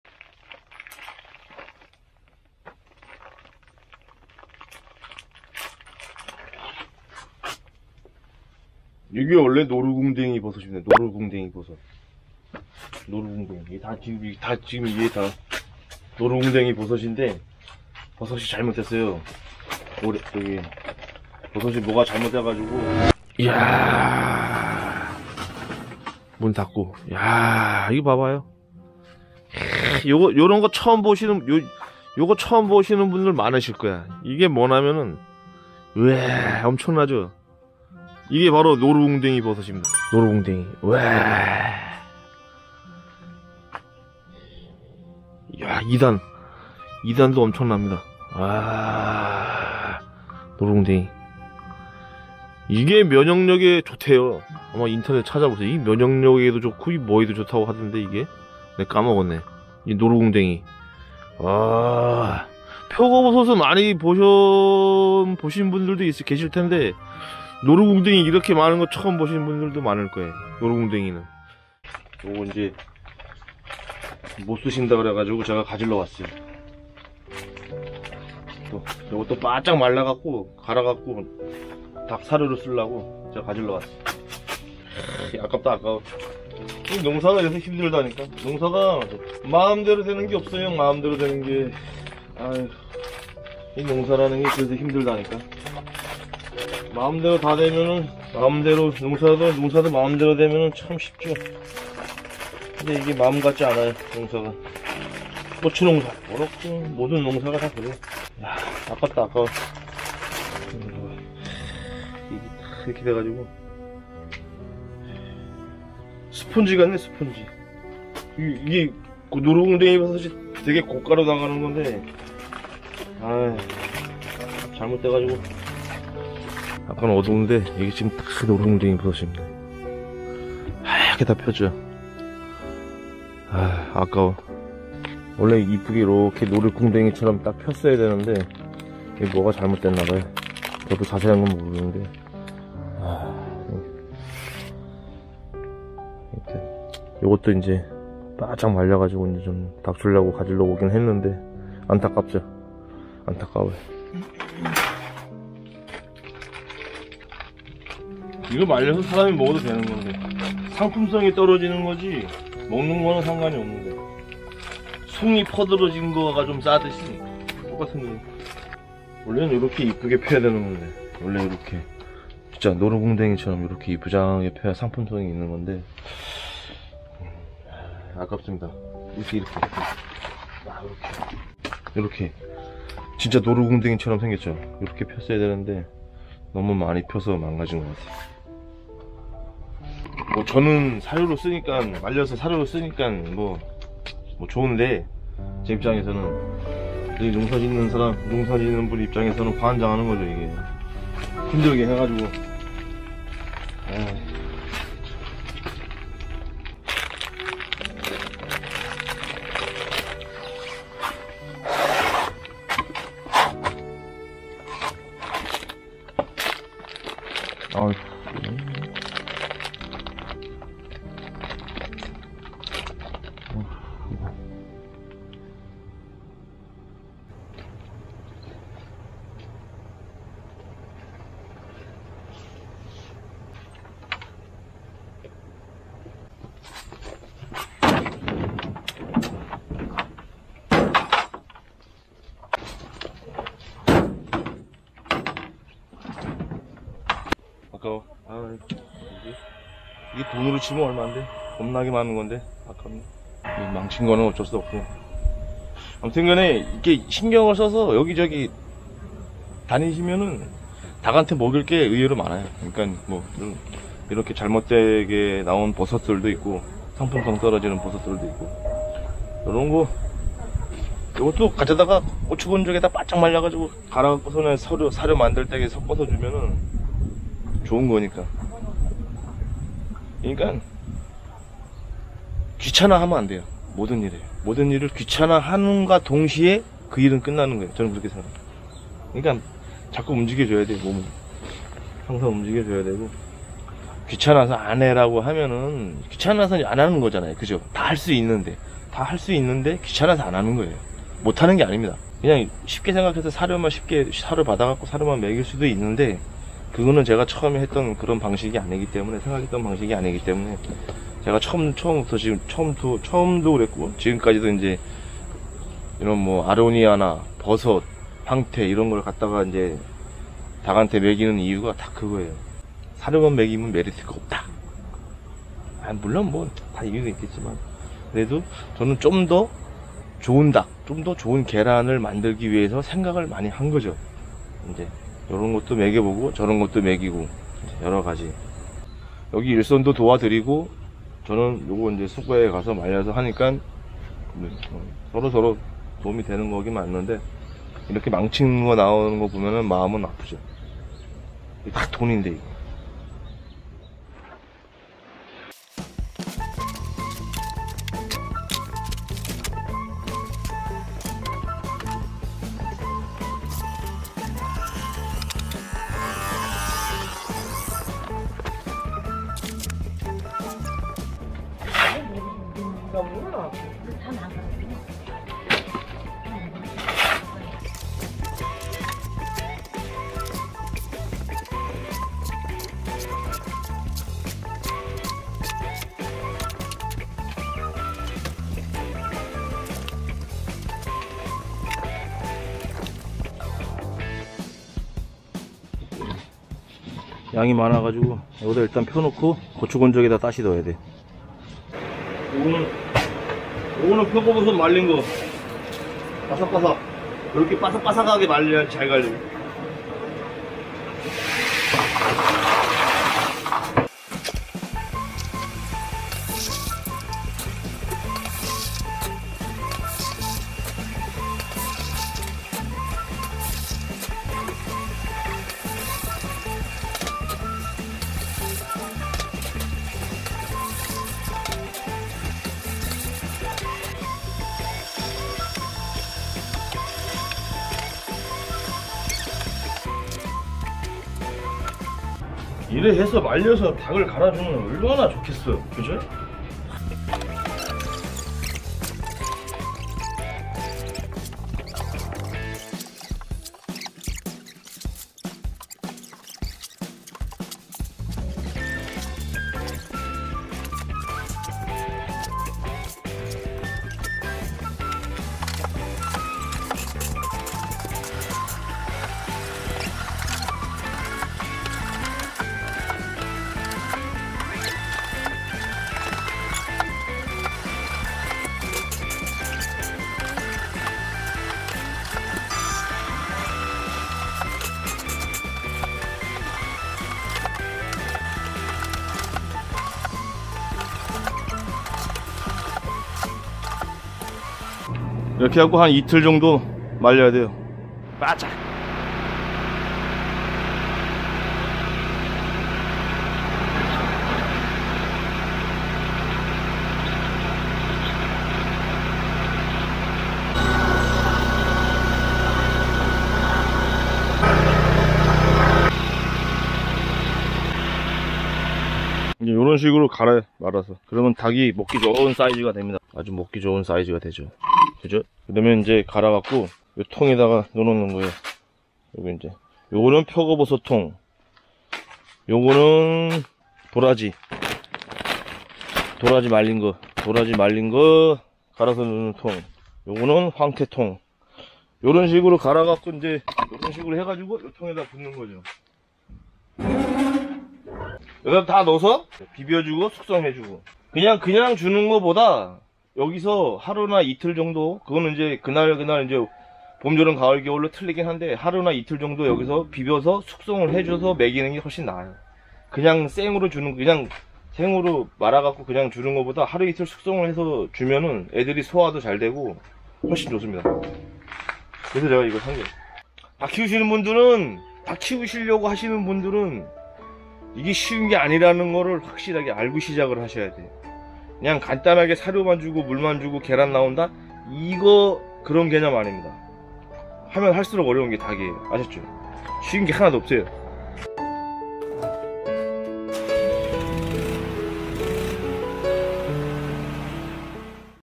대규모 노루궁뎅이 버섯 농사의 실패---농사 쉽지 않네요 ㅠㅠ-귀농한 특전사의 라이프
깊은 숲속에서 자연 방사한 닭들과 함께 살고 있는 대한민국 특전사의 라이프